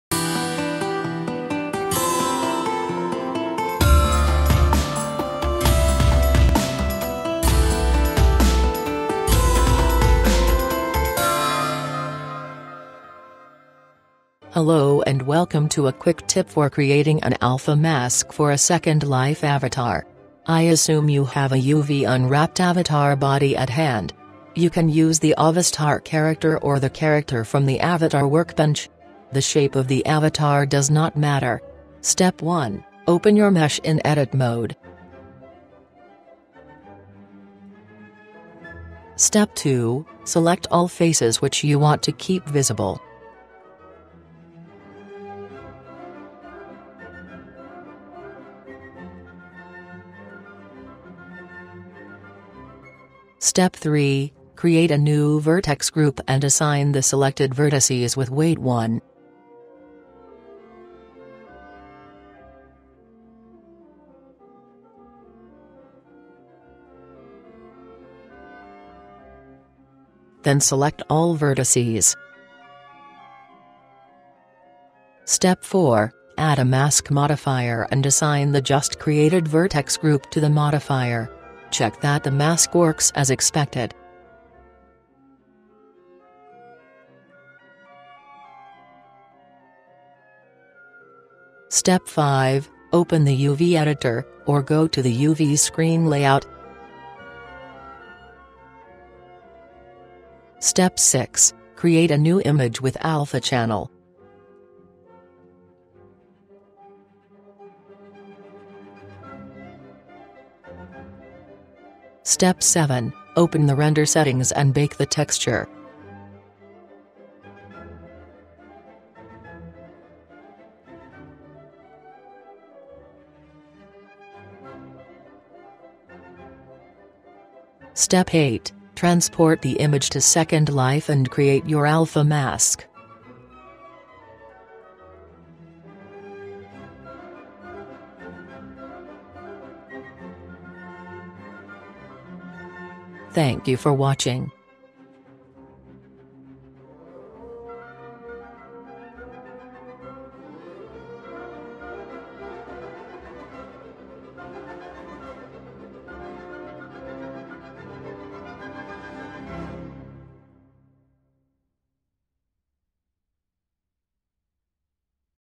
Creating Alpha Masks for the Second Life Avatar
I show you an easy way to create an Alpha mask from an existing UV unwrapped mesh. This Quicktip applies to Avastar and the Avatar workbench.